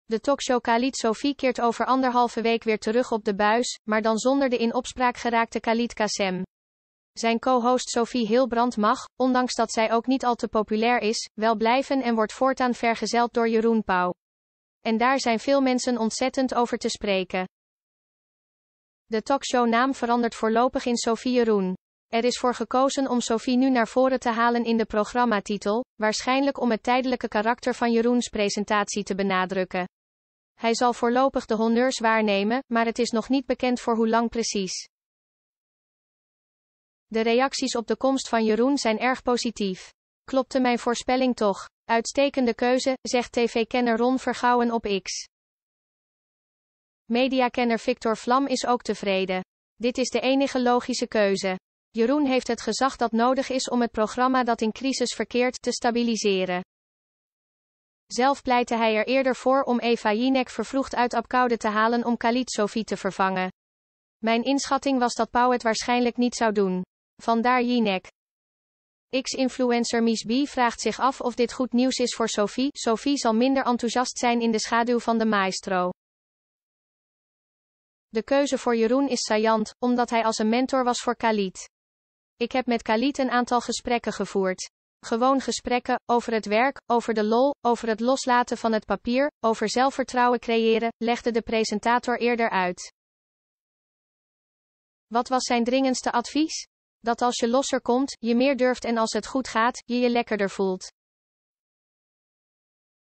Khalid Kasem vervangen door zijn mentor: ‘Pauw is goede keuze’
Khalid Kasem wordt vervangen door zijn mentor Jeroen Pauw nu zijn tv-carrière in één klap is beëindigd na schokkende onthullingen over zijn wangedrag. “Een uitstekende keuze!”